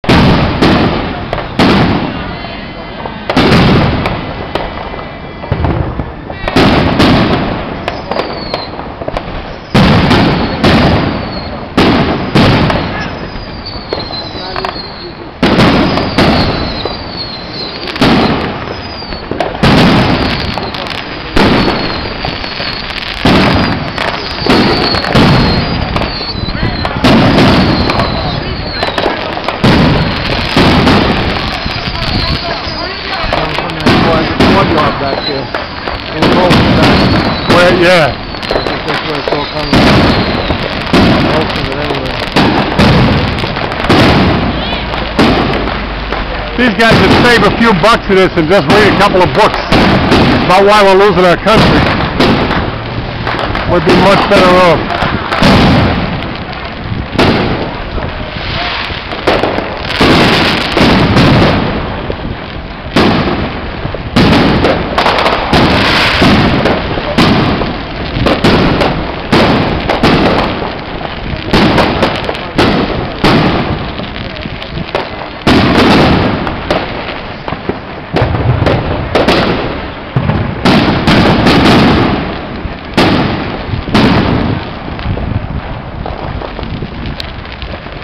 More July 4th, 2009 Street fireworks
This is what the streets in my neighborhood look like on the 4th of July. Just the local streets, mind you.